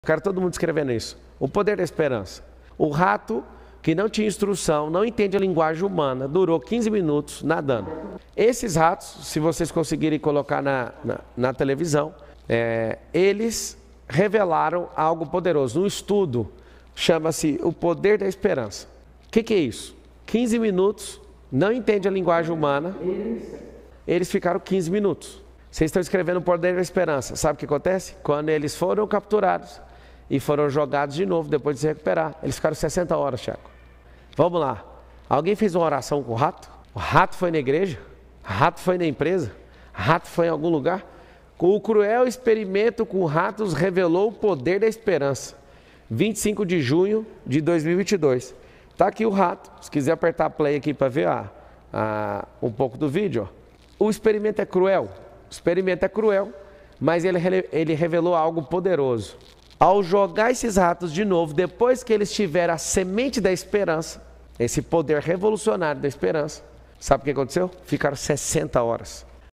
O Poder da Esperança O Experimento Cruel que Revelou um Poder Revolucionário
Eu quero todo o mundo escrever isso O poder da esperança O rato que não tinha instrução não entende a linguagem humana durou quinze minutos nadando Esses ratos se vocês conseguiram em colocar na televisão eles revelaram algo poderoso Um estudo chama-se o poder da esperança O que é isso Quinze minutos não entende a linguagem humana Eles ficaram quinze minutos Vocês estão escrevendo o poder da esperança Sabe o que acontece Quando eles foram capturados e foram jogados de novo depois de se recuperar eles ficaram sessenta horas Checo Vamos lá Alguém fez uma oração com o rato O rato foi na igreja O rato foi na empresa O rato foi em algum lugar O cruel experimento com ratos revelou o poder da esperança Vinte e cinco de junho de dois mil e vinte e dois Tá aqui o rato se quiser apertar aqui pra ver um pouco do vídeo O experimento é cruel O experimento é cruel mas ele revelou algo poderoso Ao jogar esses ratos de novo depois que eles tiveram a semente da esperança esse poder revolucionário da esperança sabe o que aconteceu Ficaram sessenta horas